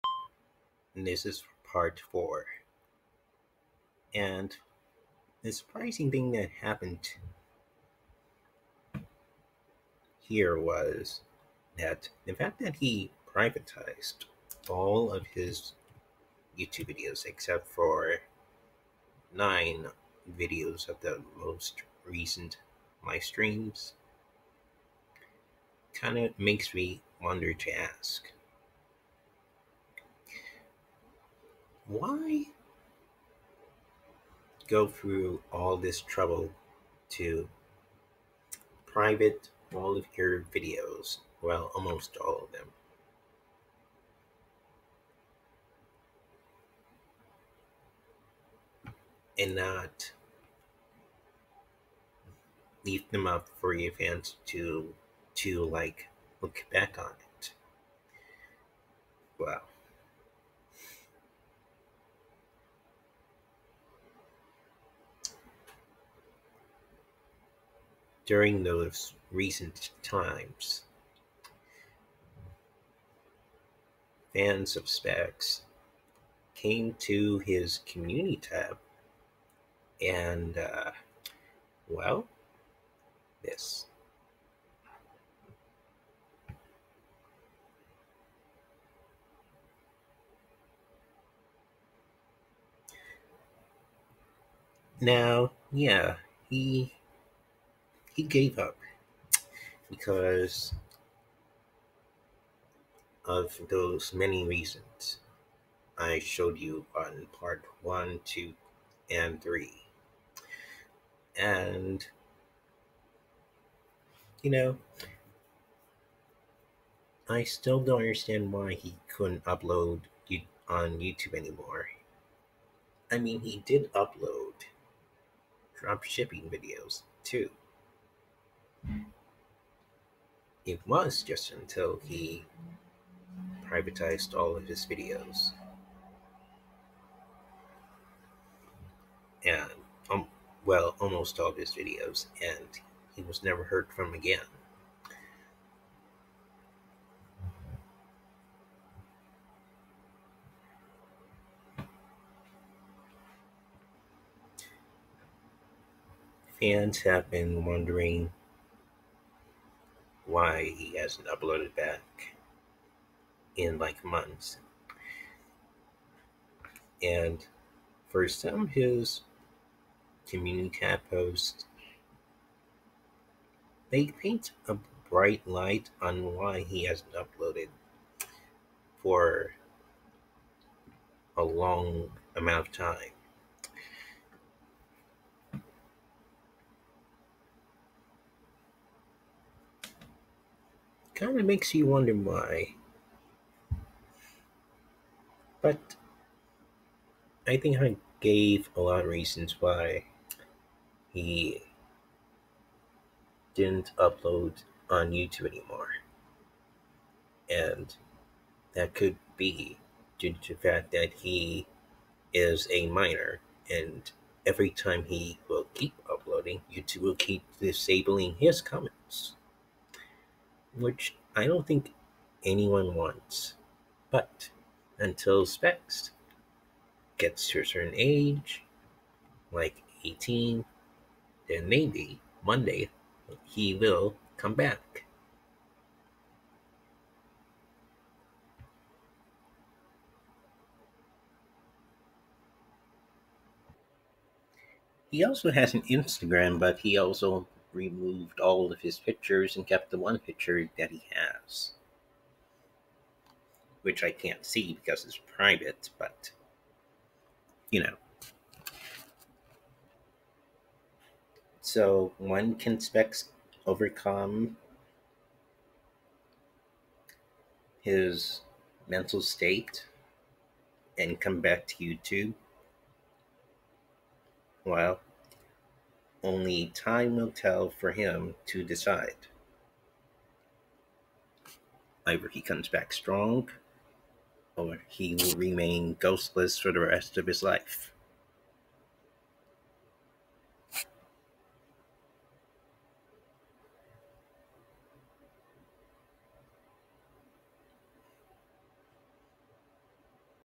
What Happened To Sphex? Part 4 | RTWRP #74
What Happened To Sphex? Part 4 |

From clickbait, to being exposed.

Sphex (Formerly Known as Sphex Games) who's real name is Parsa was a British Iranian Fortnite YouTuber with 178K subscribers. He quit YouTube to do drop shipping, the reason why is because YouTube said that he's a "minor" which to them he wasn't allowed to stream even though he is the correct age to upload videos and stream on YouTube which is a 13+ video sharing site. Since then his comment sections were also turned off by YouTube to "combat pedo comments" in 2019.

So what will happened to Sphex? Will he one day return to YouTube?